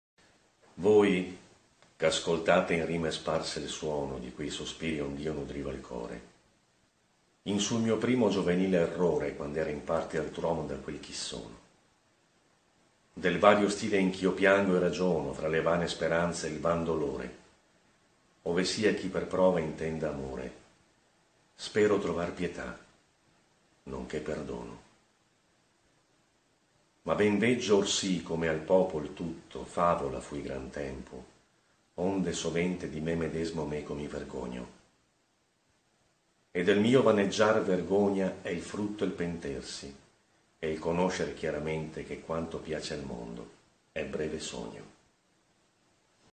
Francesco Petrarca, proemial sonnet of Canzoniere (making of)
This short video shows the "making of" of the video: "Francesco Petrarca, proemial sonnet of Canzoniere". The facial MoCap has been performed thanks to the free and open source software Blender (within the distribution ArcheOS).